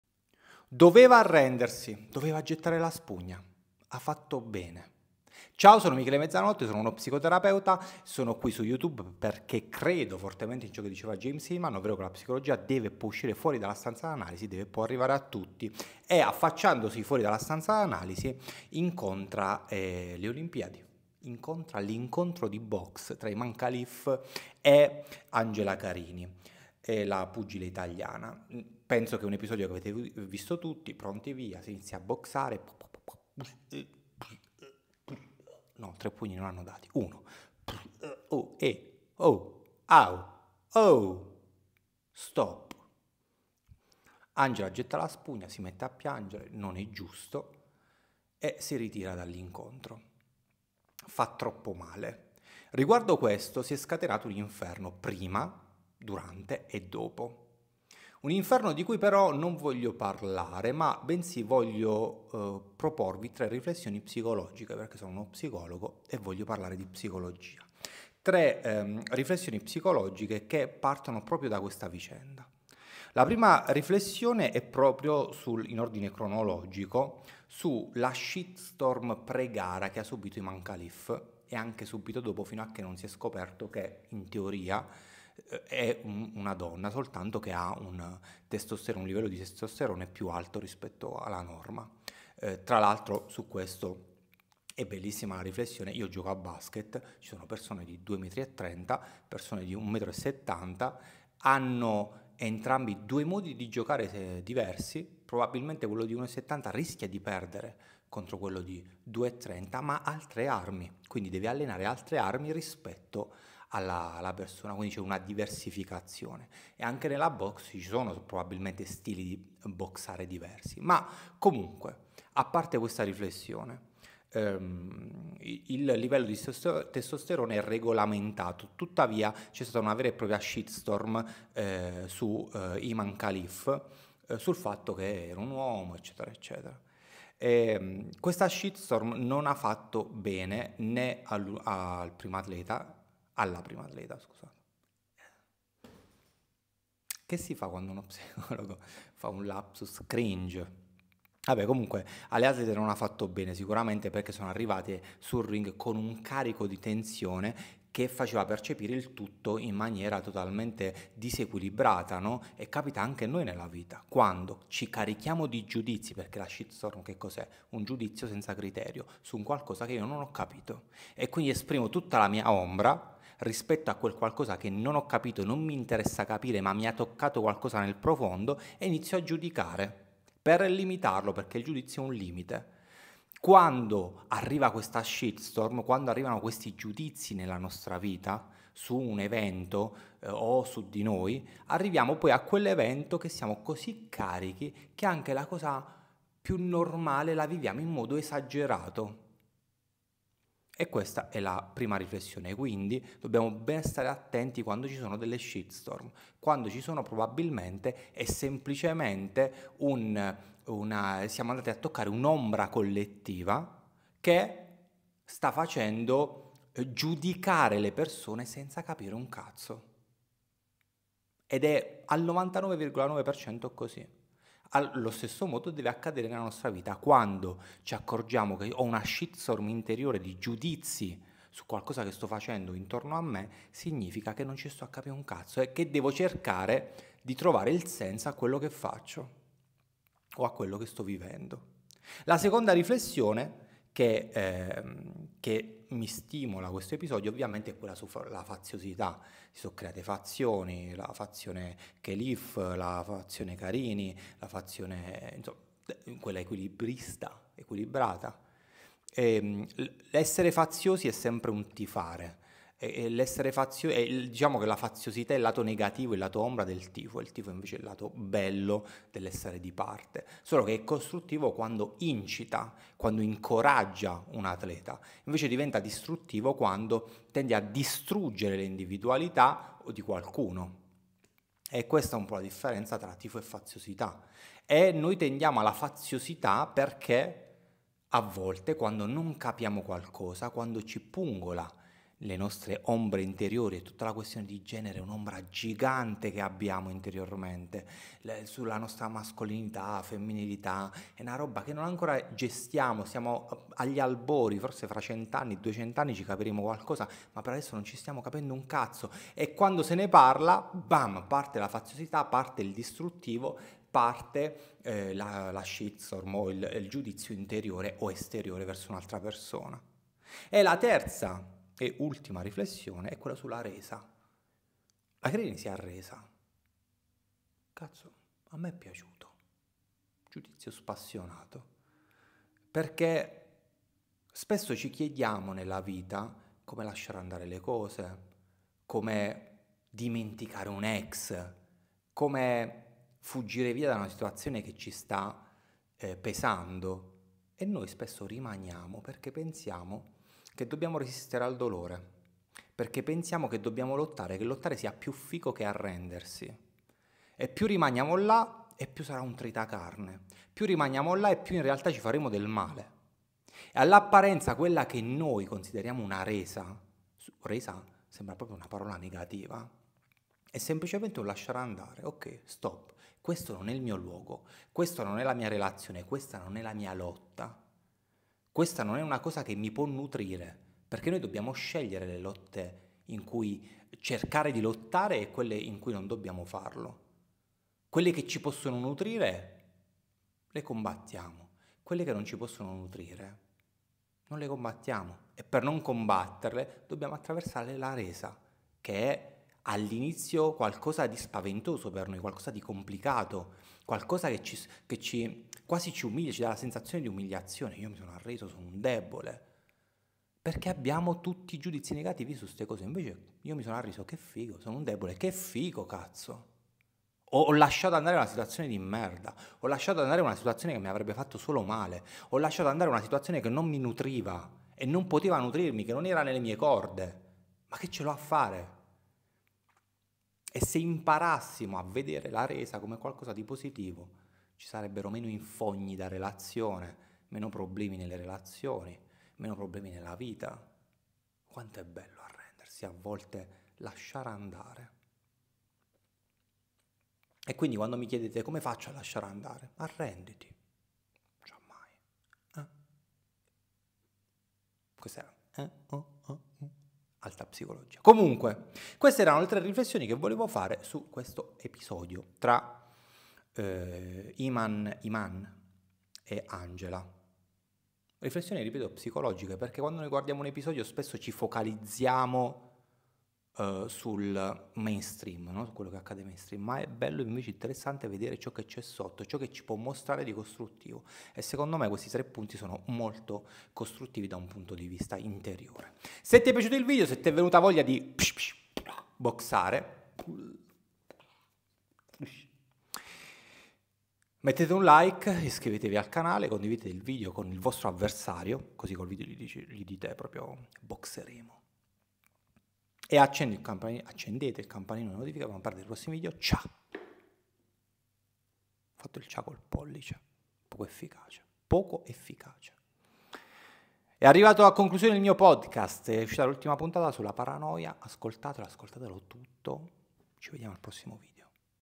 Imane Khelif e Angela Carini: tre riflessioni psicologiche
Tre riflessioni psicologiche sull’incontro tra l’azzurra e l’algerina